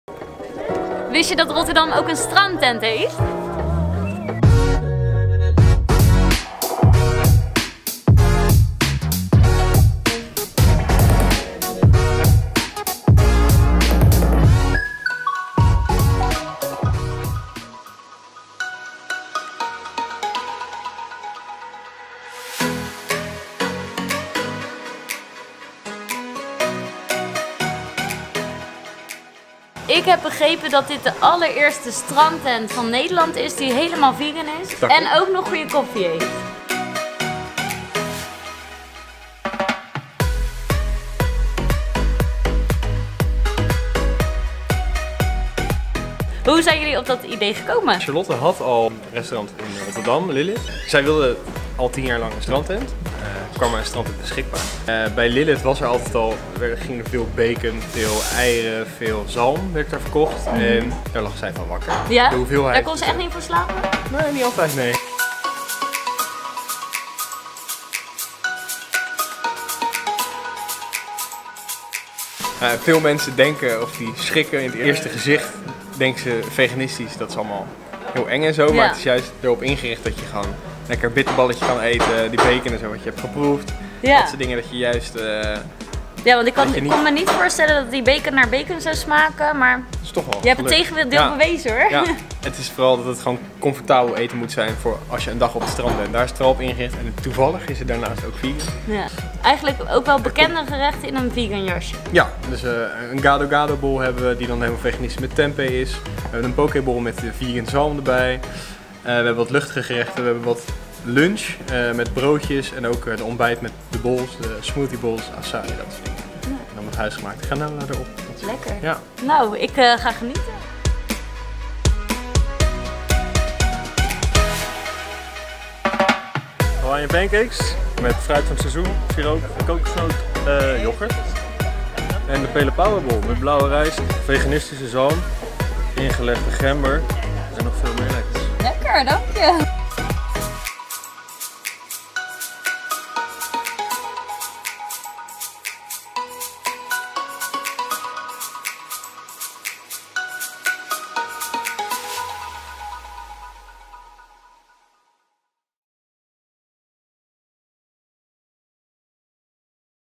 DE EERSTE VEGAN STRANDTENT IN NEDERLAND | HOTSPOTTERS xNina
Het strand, lekkere vegan gerechten en goede koffie: het komt bij Pele Surf Shack allemaal samen. In deze vlog gaat Nina langs bij de allereerste vegan strandtent van Nederland! 🏖